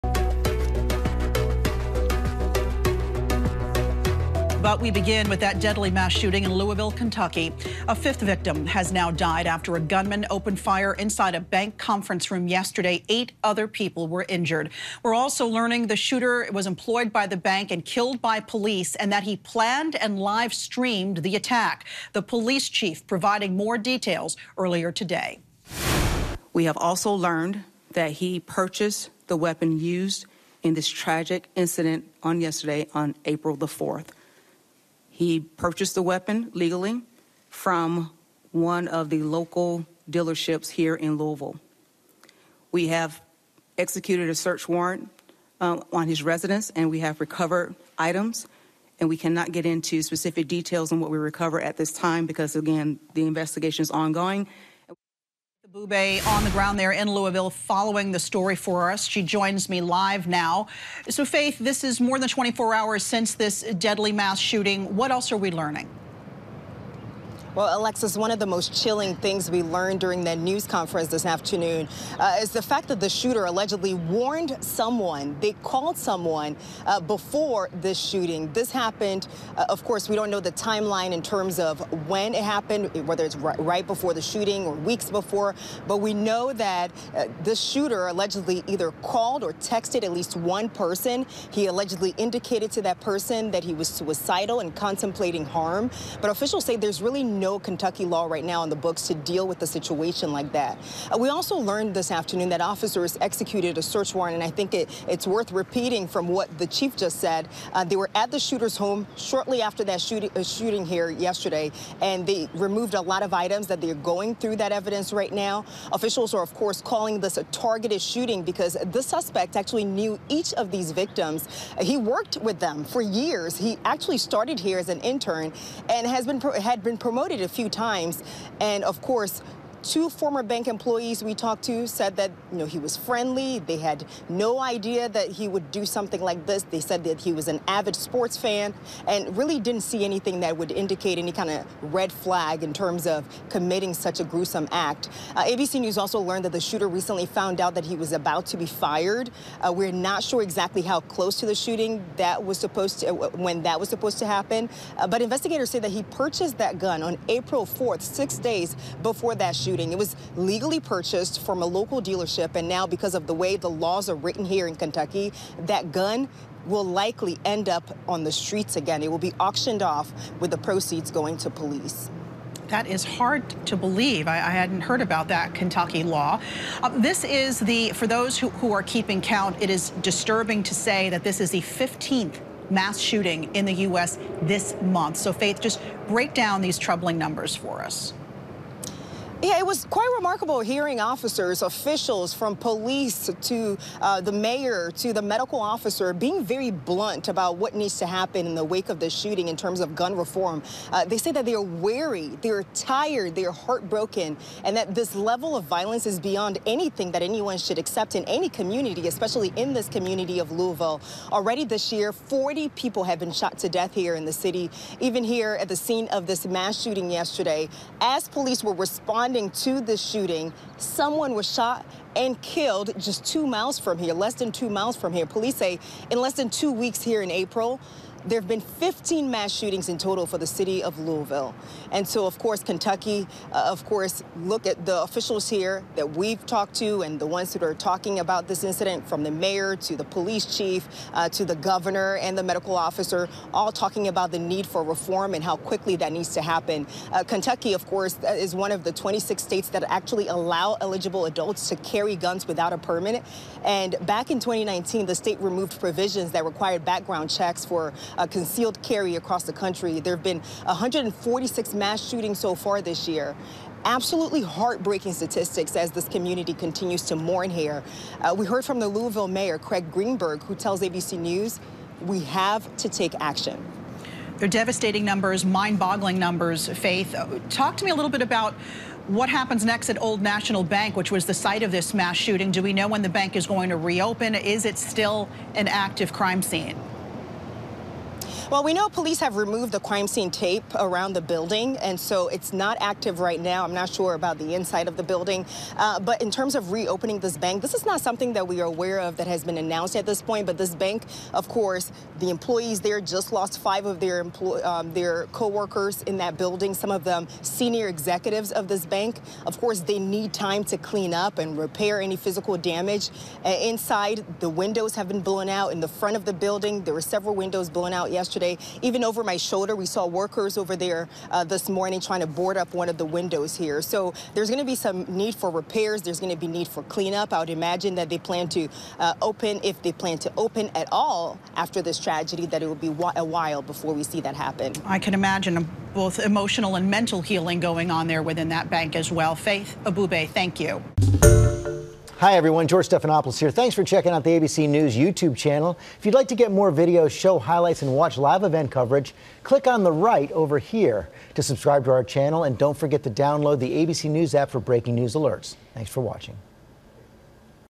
Louisville gunman planned and livestreamed attack: Officials
The shooter legally purchased a gun at local retailer, killing at least five people.